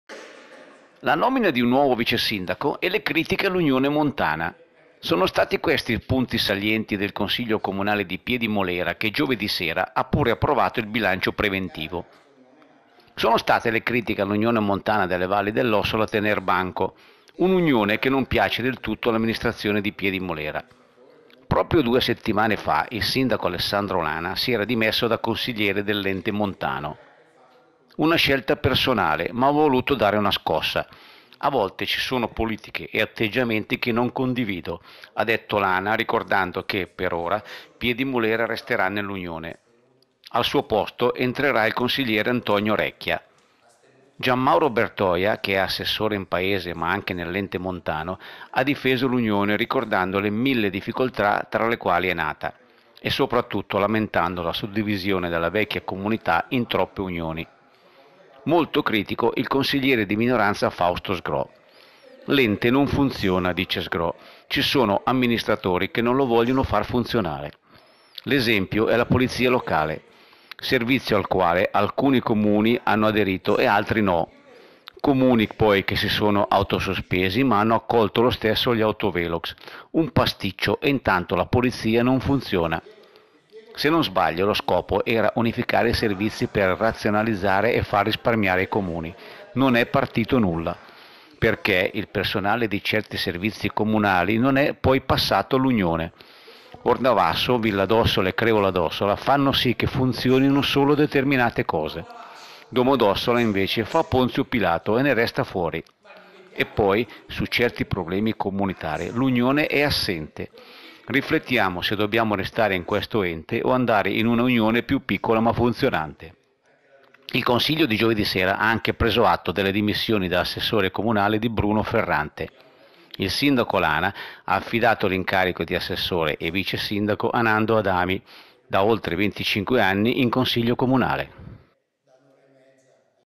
Piedimulera, nuovo vicesindaco e critiche all'Unione montana
24 febbraio 2017 - Piedimulera, nuovo vicesindaco e critiche all'Unione montana in consiglio